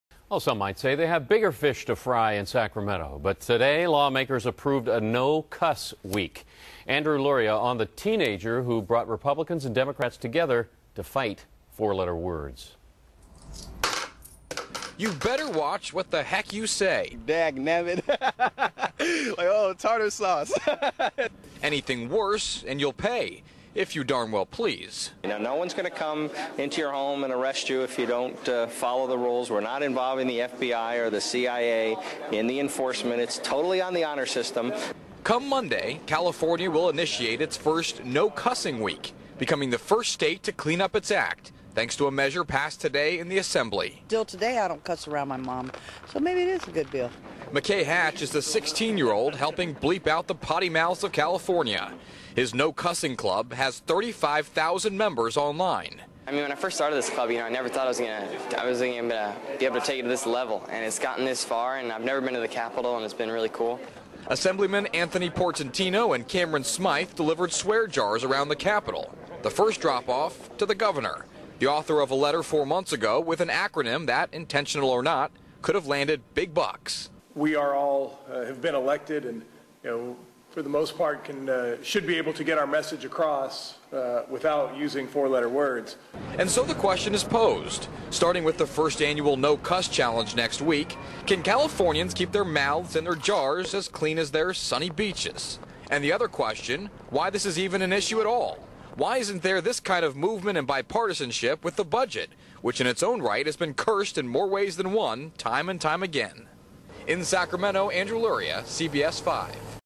California Assembly passes "No Cuss Week" legislation.
You'd better watch what you say.  California Assembly approves "No Cuss Week" legislation.  No really.